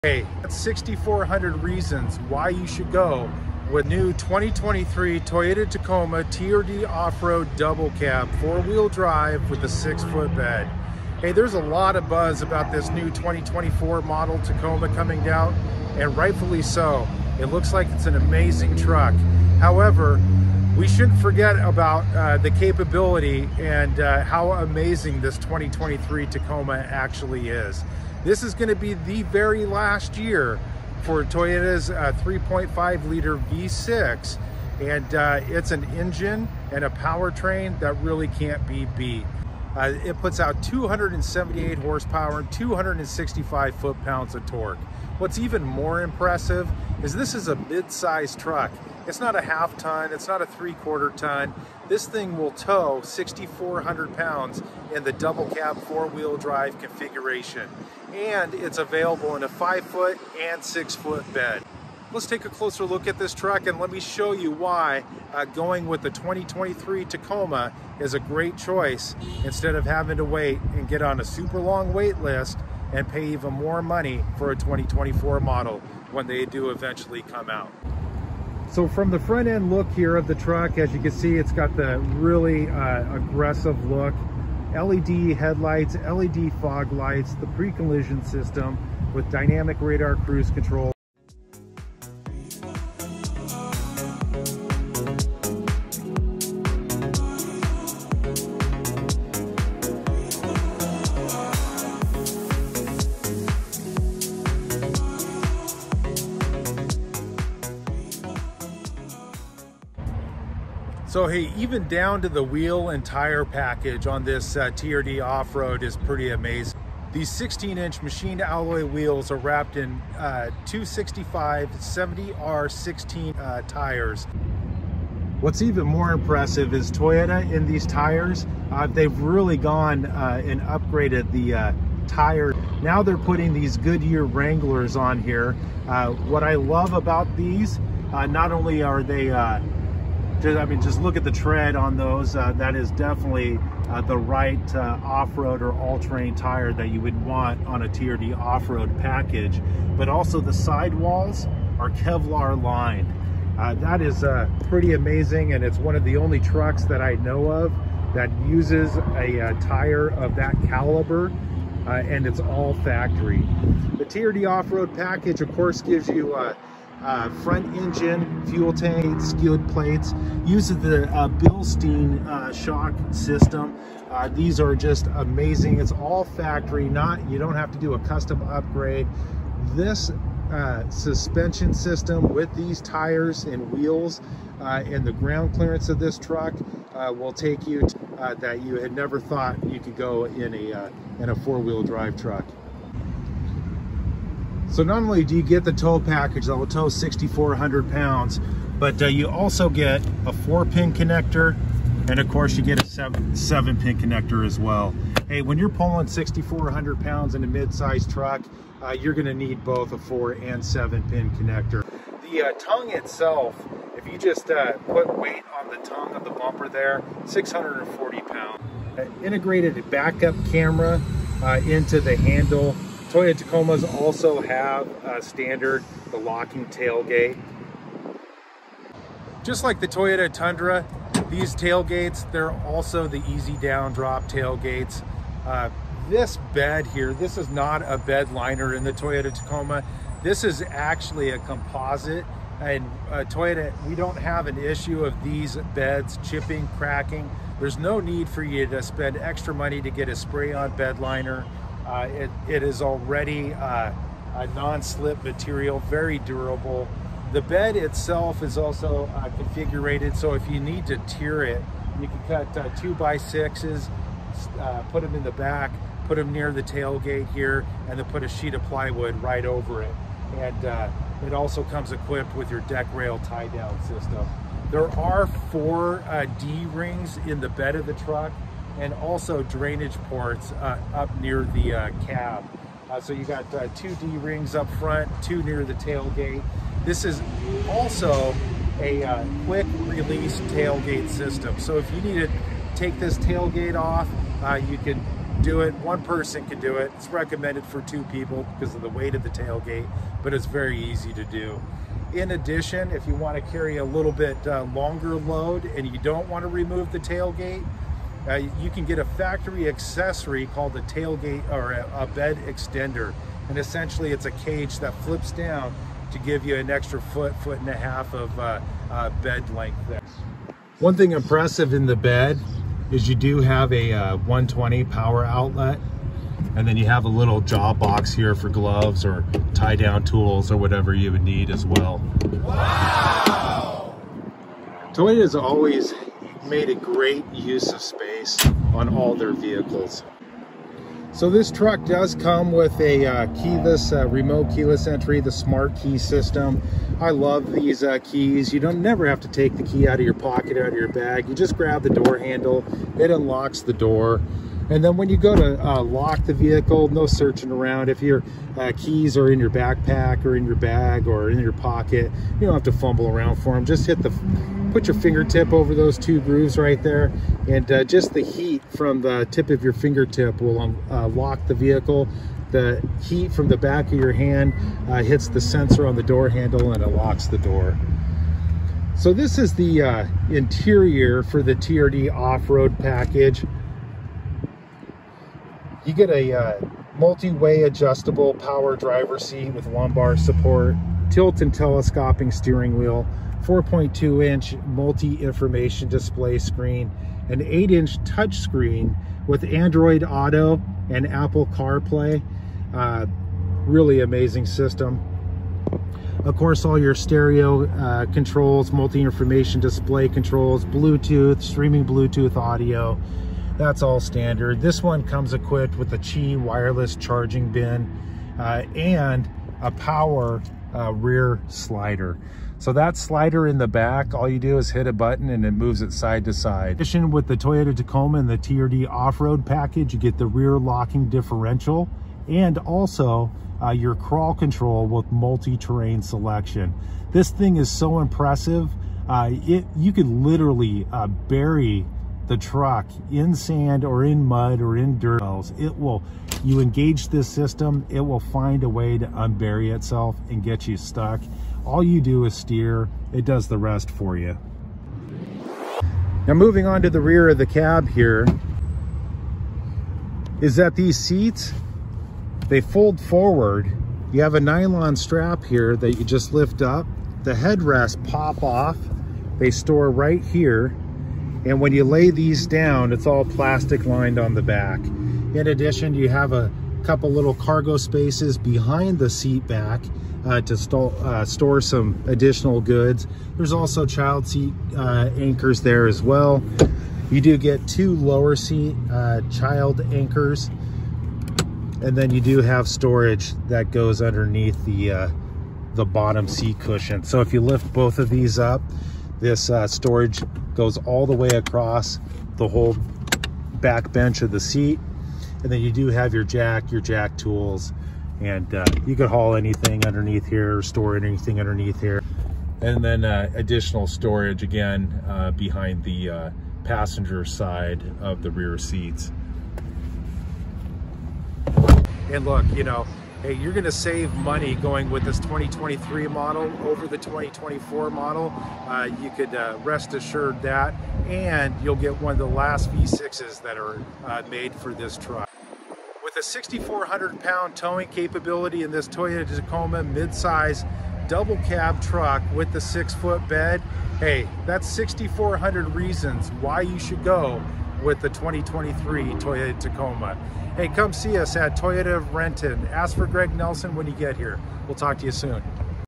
Last Year For The V6 - 2023 Toyota Tacoma TRD Off Road Double Cab 4x4
You'll give up on this truck before it gives up on you. Check this out I’ll give you 6400 Reasons to buy the 2023 Toyota Tacoma TRD Off Road 4x4. It's the last year of that Toyota will be using the V6 in the Tacoma. Soon to be replaced by a turbo 4 cylinder or on the higher end models a turbo 4 cylinder hybrid. The Tacoma has long been the leader in the mid-size truck market. Although this current model has been refreshed over the years and in 2016 the 4.0lt V6 was replaced by the 3.5lt Atkinson style engine, it's pretty much been the same crazy reliable truck since 2005. Look if you want a solid, reliable truck with great towing and amazing off-roading capabilities and a V6, get the 2023 Toyota Tacoma 4x4.